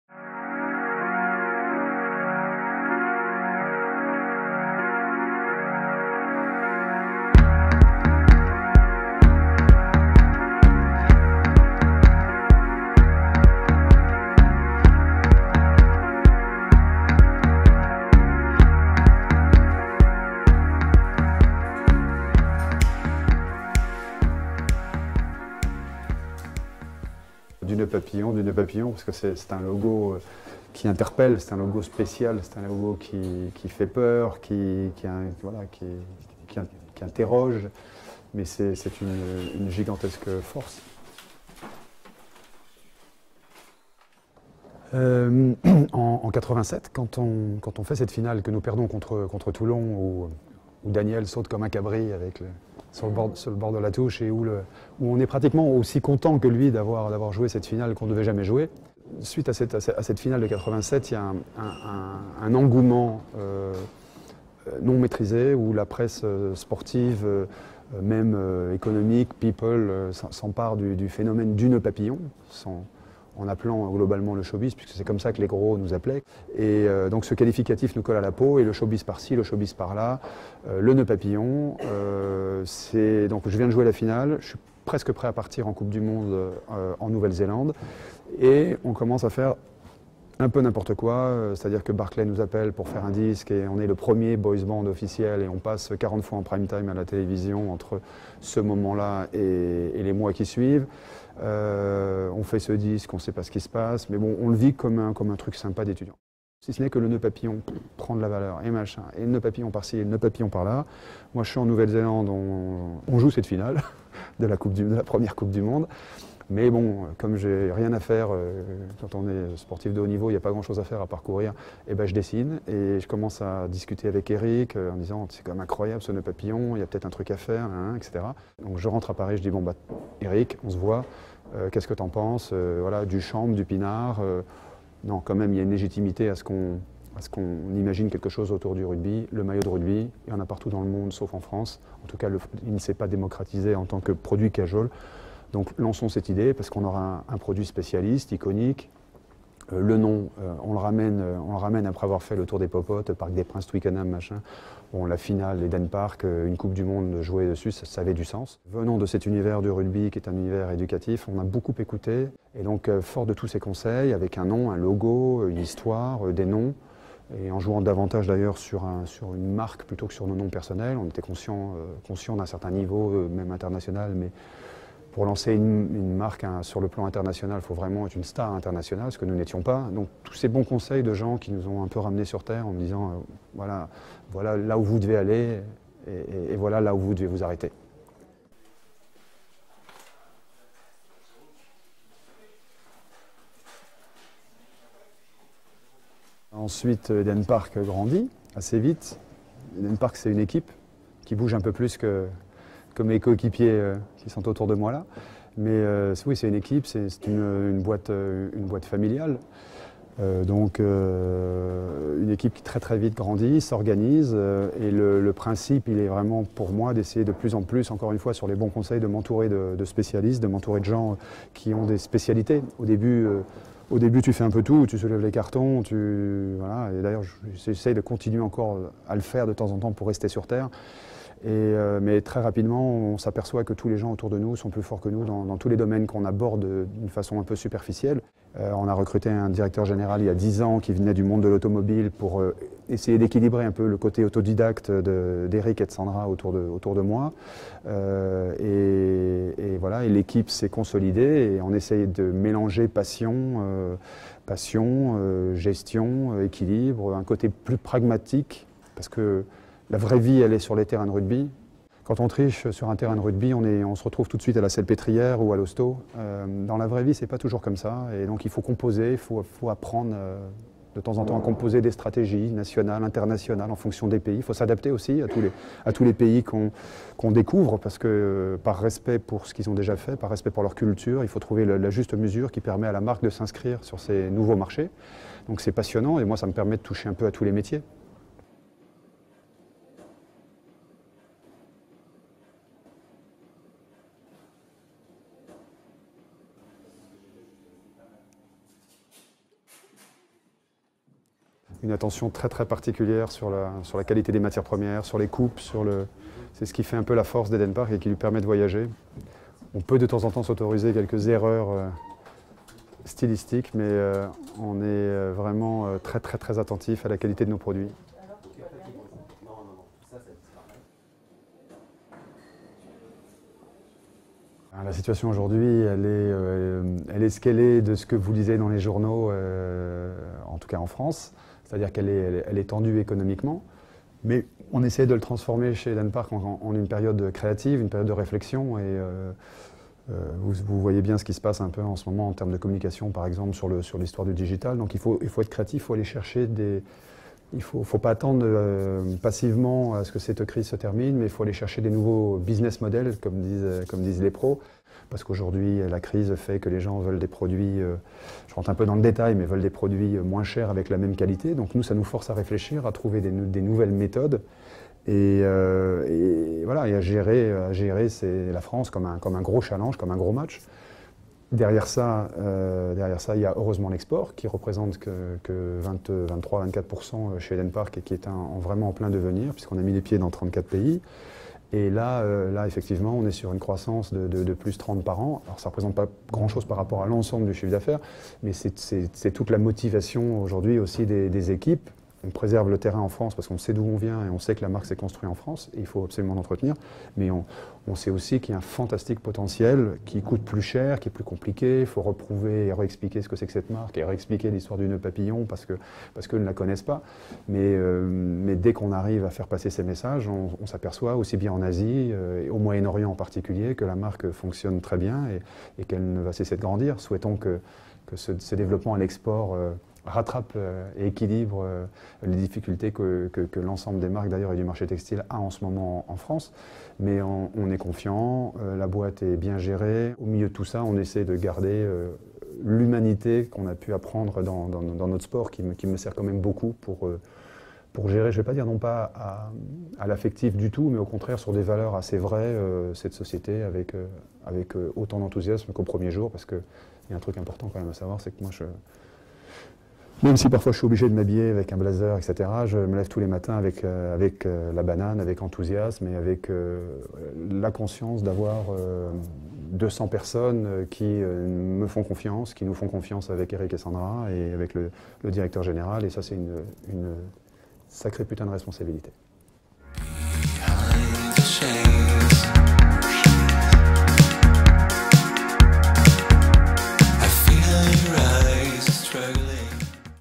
Ils sont Barbarians : Quand Franck Mesnel raconte l'histoire du noeud papillon
La genèse d’une impertinence et la culture du French Flair. Quand l'ancien Racingmen et Barbarian Franck Mesnel raconte l'histoire d'Eden Park et du noeud papillon...